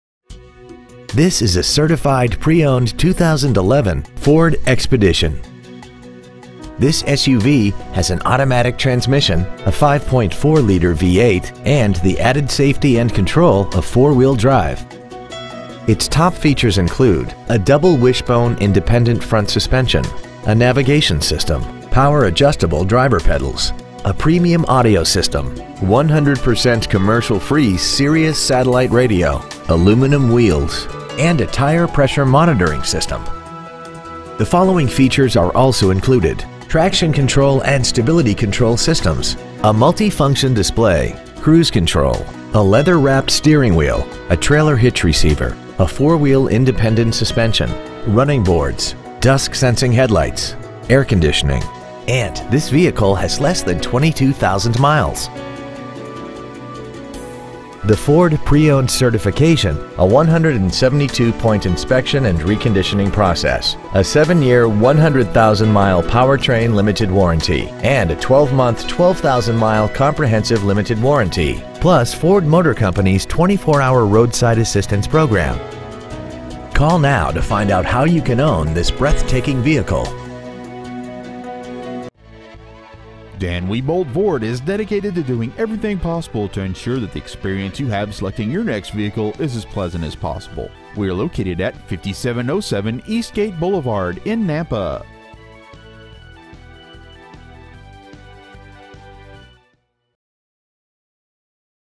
Pre-Owned 2011 FORD EXPEDITION Nampa ID
Year : 2011
 Make : FORD
 Model : EXPEDITION
 Engine : 5.4L V8 24V MPFI SOHC FLEXIBLE FUEL
 Trans . : 6-SPEED AUTOMATIC
 Exterior : OXFORD WHITE
 Miles : 21,557

 Stock : 2A1352

 5707 East Gate Blvd.